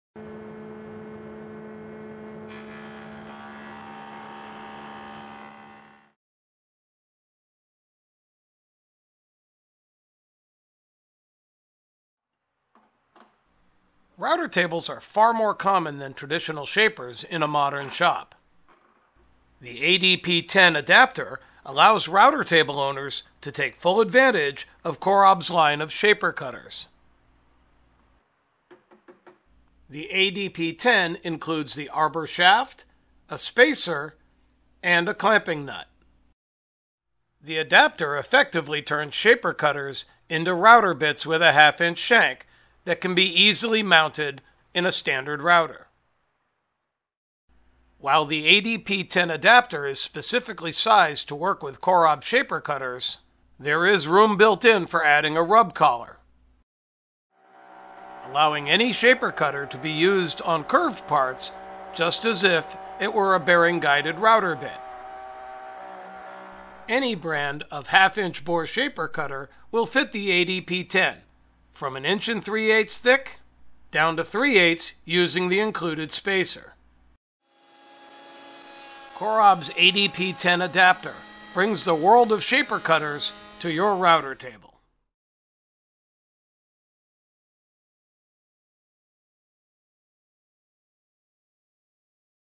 ADP 10 Adapter for 1/2" Bore Cutters
This precision arbor allows for using Corob's shaper cutters in your router table. It has enough capacity to accept rub collars and works with any 1/2" bore shaper cutters up to 1-3/8" tall.
Best of all, it means that you can take advantage of Corob's exclusive matching profiles for both molding head knives and shaper cutters.